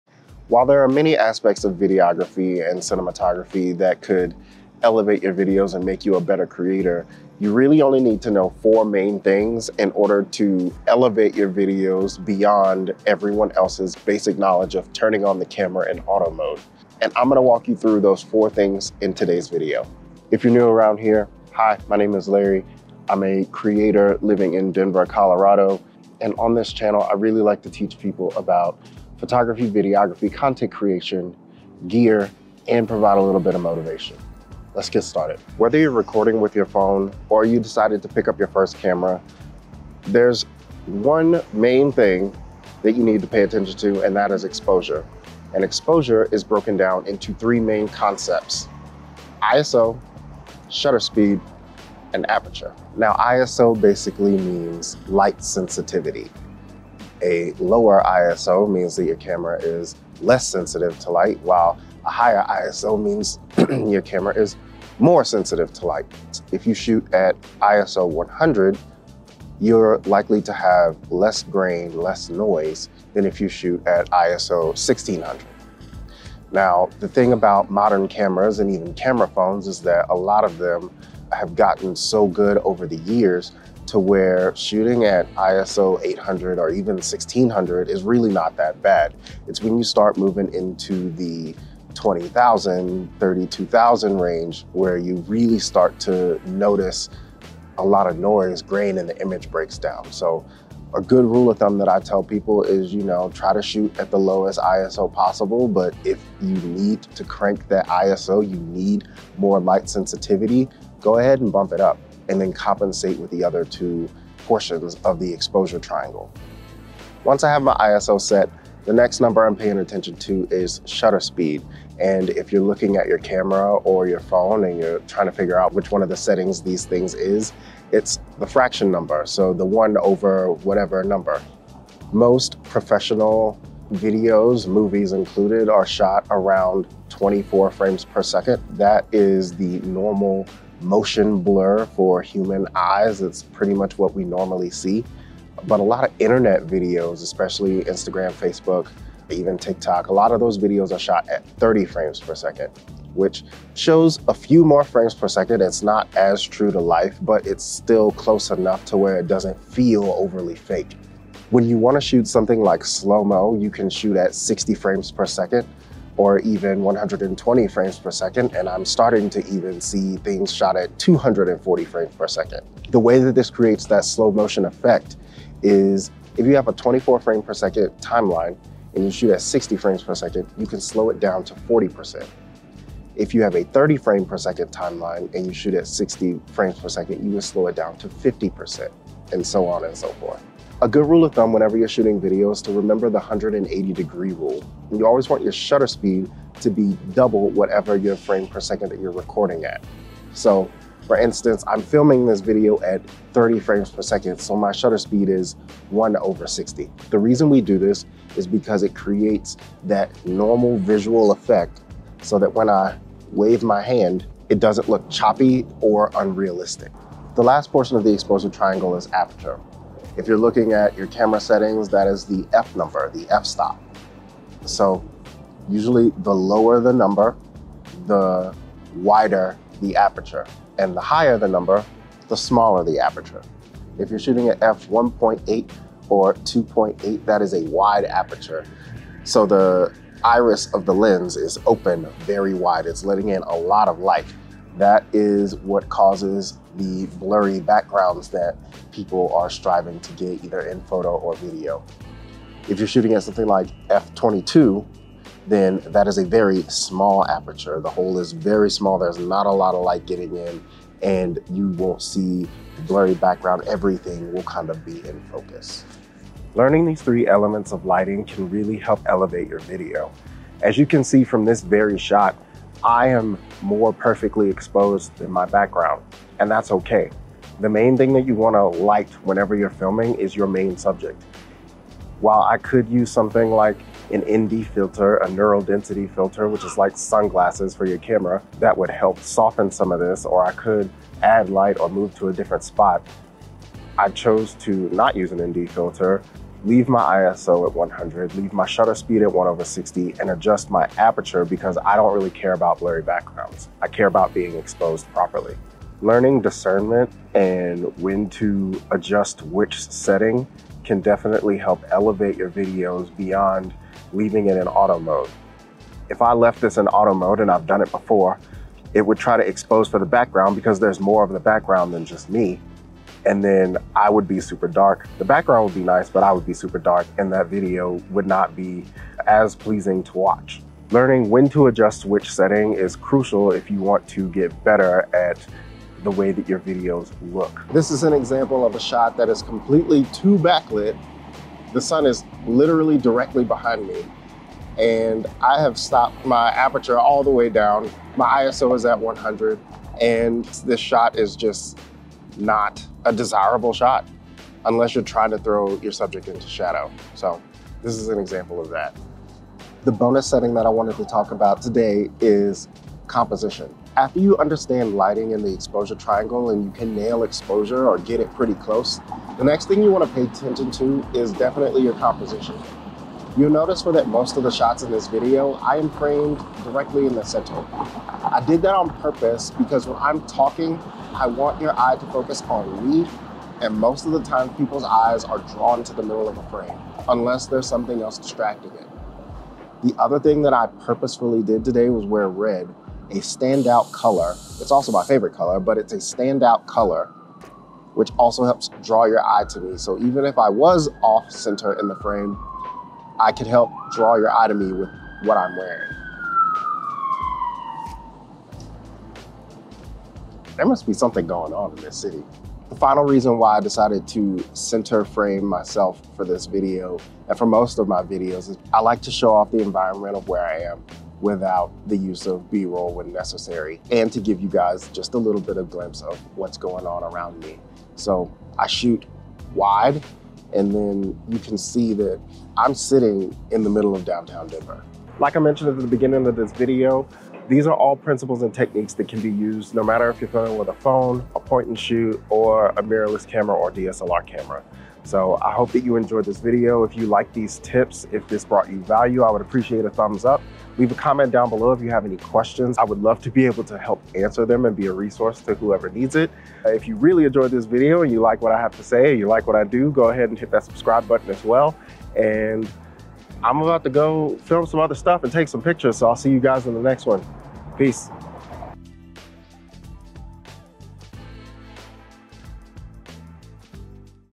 The Exposure Triangle: A Beginner's Guide to Photography and Videography
Mastering the principles of exposure will elevate your photography or videography and allow you to actually create specific looks without relying on a camera's algorithm.

Once you master exposure, composing your images with purpose will make your content leaps and bounds above those who remain on auto and perform without purpose. Whether you're using a DSLR, mirrorless camera, or even a smartphone, understanding the relationship between aperture, shutter speed, and ISO is essential for capturing stunning images and videos.